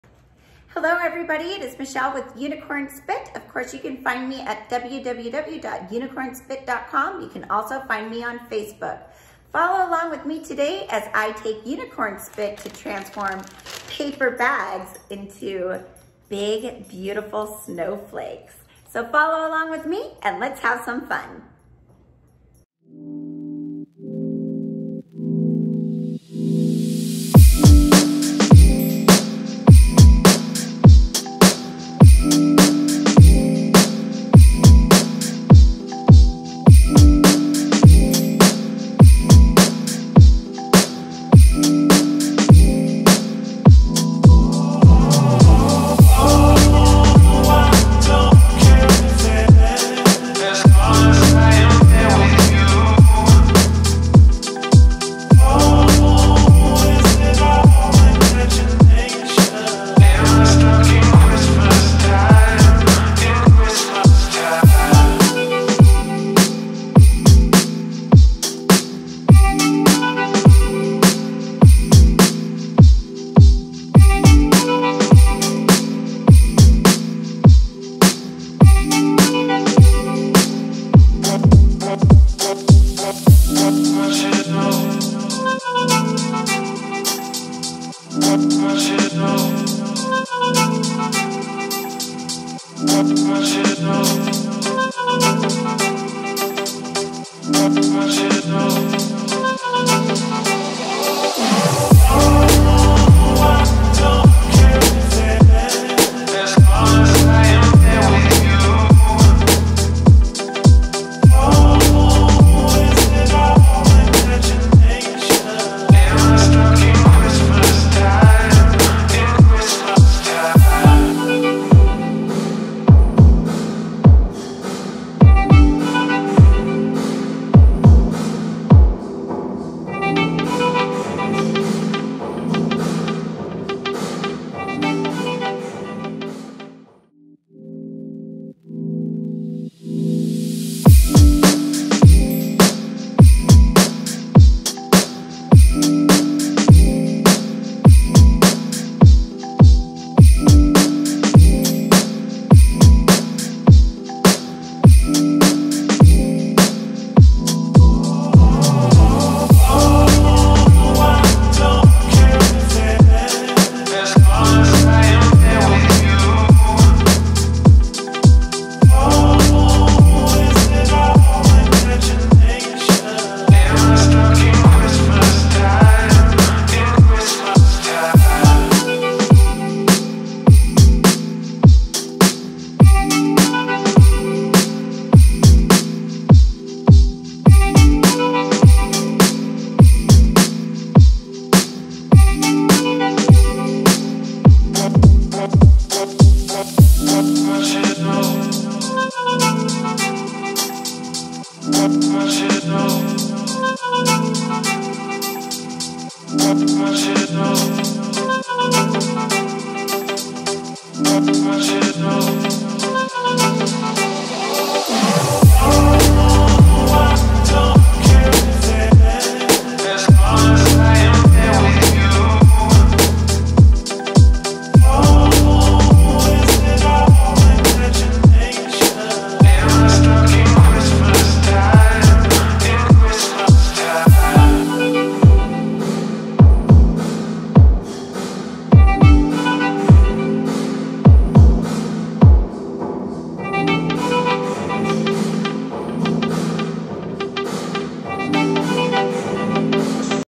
3D Paper Bag Snowflakes - Colorful
Follow along as Michelle Nicole turns paper lunch bags into a beautiful piece of watercolor art!
These are the perfect family craft on snow days to prepare for the winter festivities & Christmas.
A unique spin on old fashion paper sack snowflakes.

This project is great to do with kids. There is no need to burn the edges. It was only done to add a dramatic, artistic look.

Audio claim
Video & audio created via - SPLiCE iOS App with Paid Annual Membership including all music & editing tools offered in app.